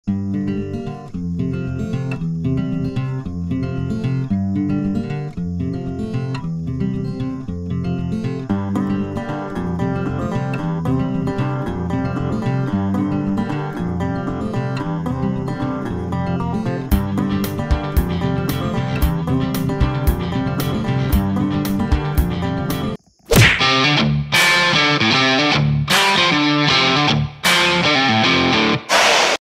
SHOOT or PASS? | Pennsylvania WHITETAIL BUCK | HUNTING
Here’s a youtube short of a pretty PA buck! If you lived in Pennsylvania, would you take a shot at this buck or pass him up? What state are you from & would he be a shooter there?